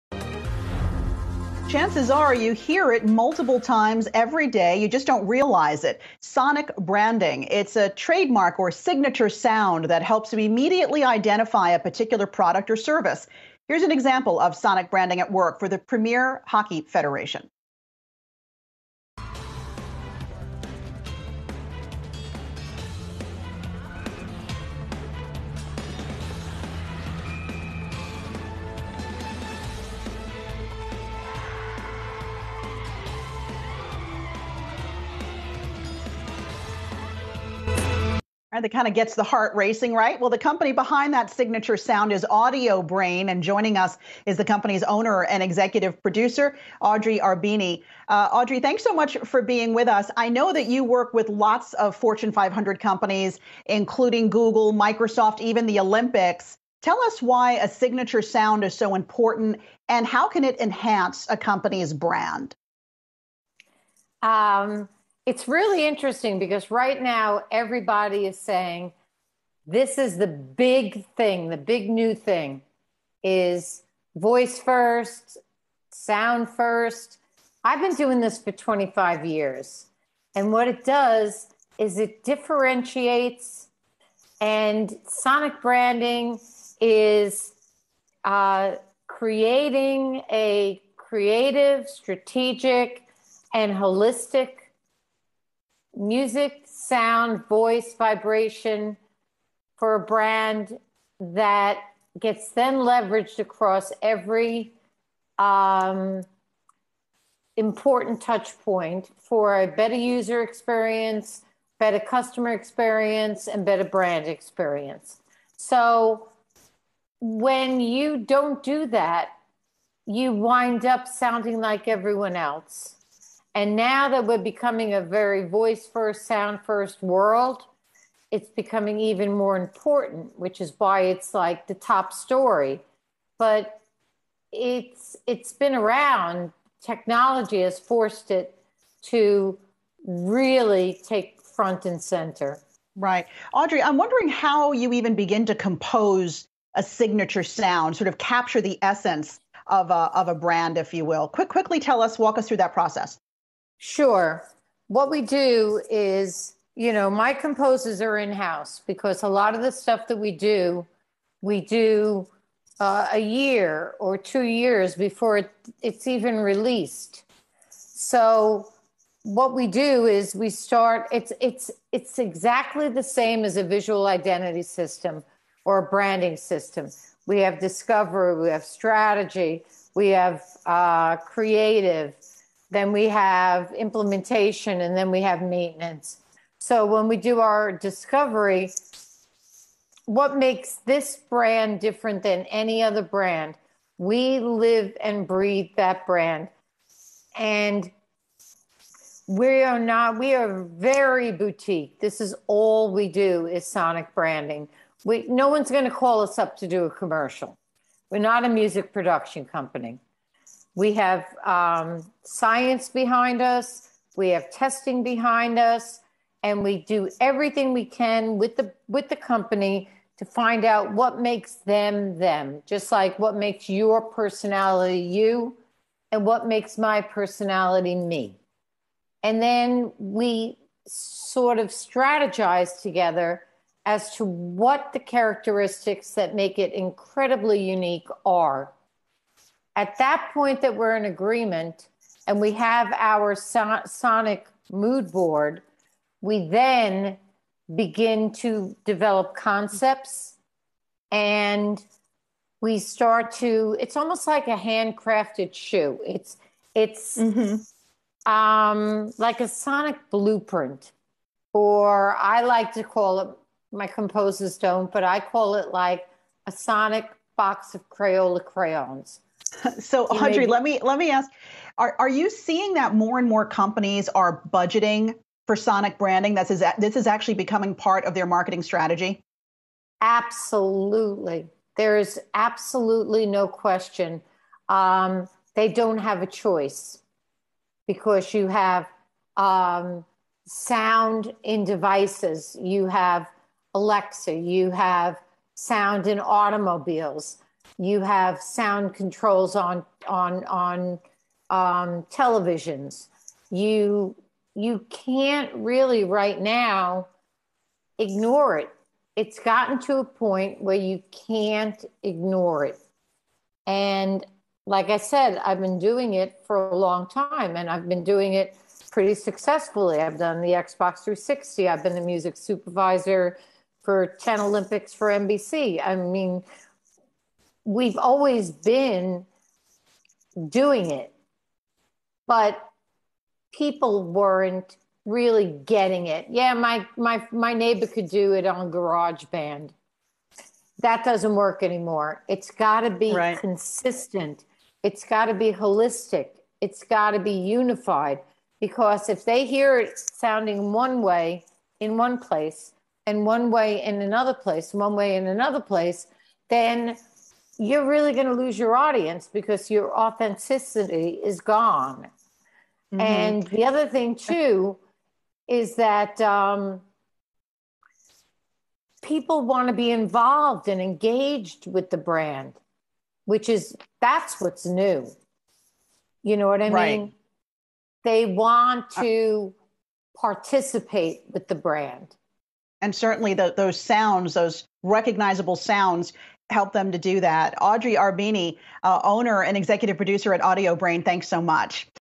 The art of Sonic Branding and how it can enhance a company's brand
Audiobrain Owner and Executive Producer Audrey Arbeeny joins Yahoo Finance Live to discuss "sonic branding" and how signature sounds can help elevate brands.